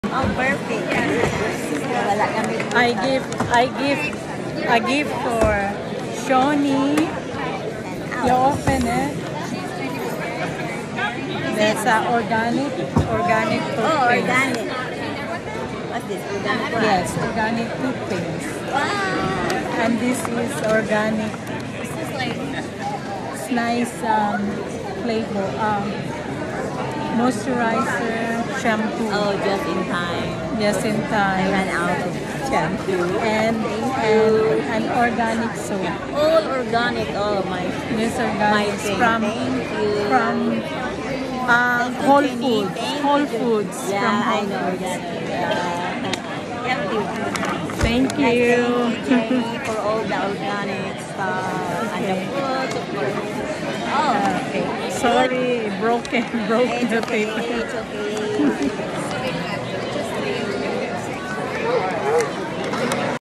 Im giving Sionne a gifts 🎁 before shes going to California USA 🇺🇸
100 💯 years anniversary  celebration  Our lady of perpetual help church ⛪️ June 25,2023